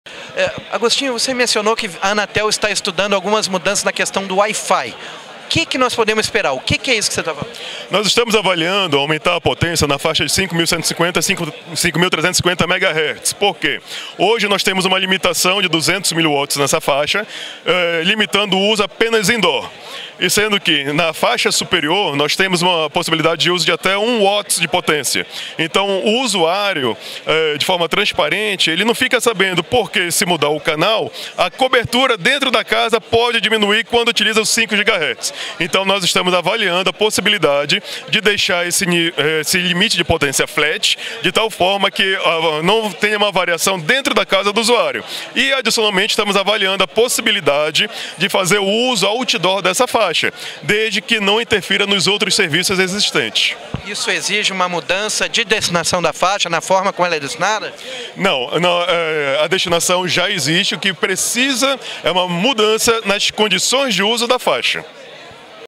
Anatel vai permitir que roteadores WiFi tenham potência maior
“Hoje temos uma limitação de 200 miliwatts na faixa de 5150 a 5350 MHz, limitando o uso apenas indoor, sendo que na faixa superior temos possibilidade de uso de até 1 Watt de potência. Plano é deixar esse limite flat”, explica o gerente de espectro da agência, Agostinho Linhares.